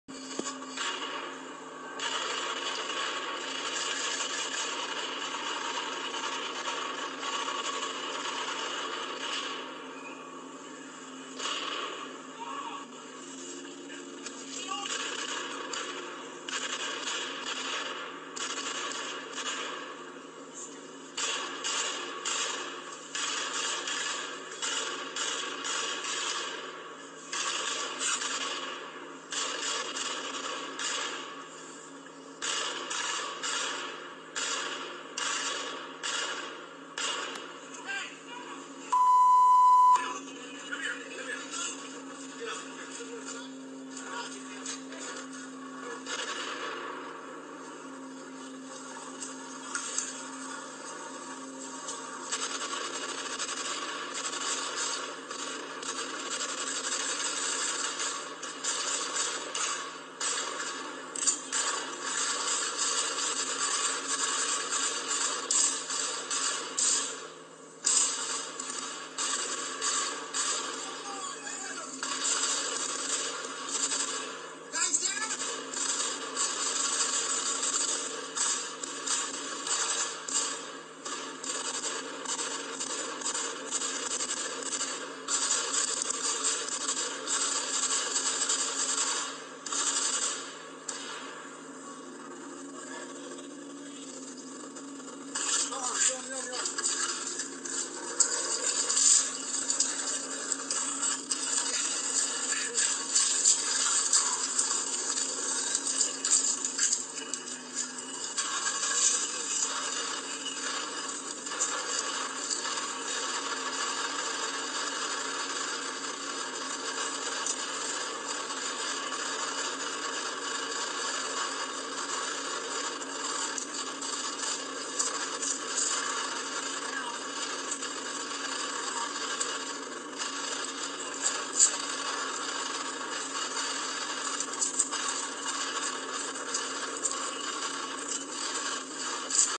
VIDEO of Dorner Shootout w/Police
Dorner Shootout video. Dorner police Shootout video. Video of Dorner shootout, includes audio from "news" story from chopper that included audio ...that makes the impression of contact with Dorner as part of the shoot-out. I don't seen Dorner in the shootout video